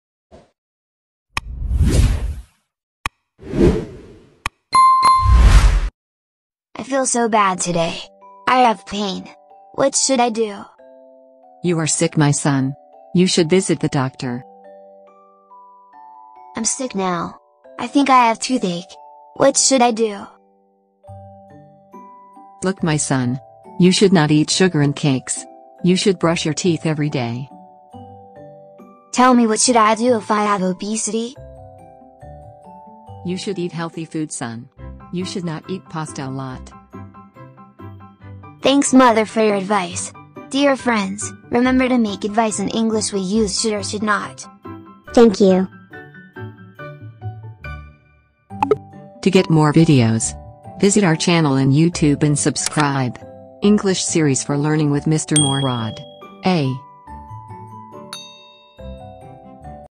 Making advice
come and see our latest video about how to make advice in English using should and should not.